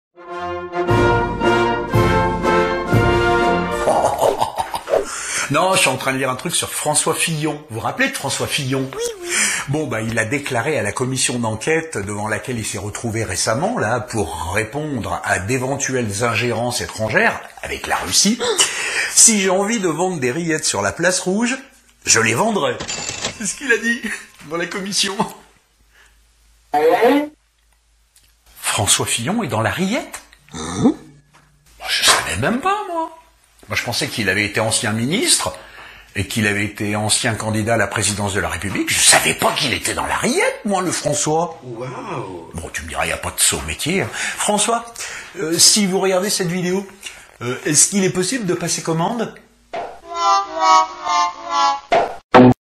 François Fillon cachait bien son jeu !!
L'ex ministre, l'ex candidat à la présidence de la République devant une commision d'enquête.

Merci d'avance  ;-)